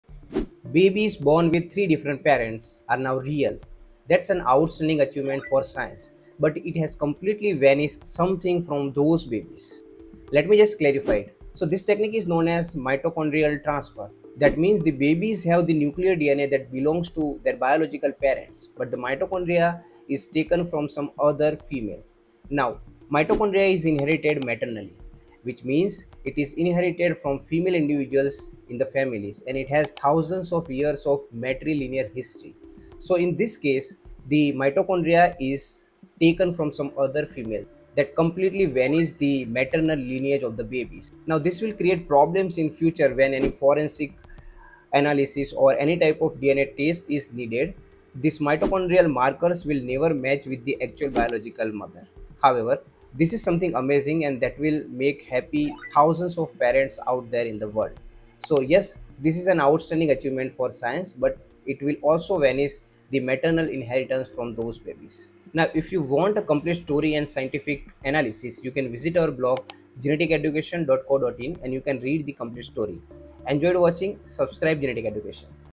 Three-Parent Babies: A Breakthrough That Ends One Ancestral Lineage
Mitochondrial donation (three-parent IVF) is saving babies from deadly genetic diseases — but there’s a twist. By replacing a mother’s faulty mitochondria with that of a donor, this technique erases the maternal mitochondrial lineage forever.

What does this mean for future generations?

No inherited mitochondrial DNA from the mother.

A permanent break in one genetic line.

Ethical and evolutionary questions for humanity.

Watch to learn how this life-saving technique rewrites our ancestry and genetics.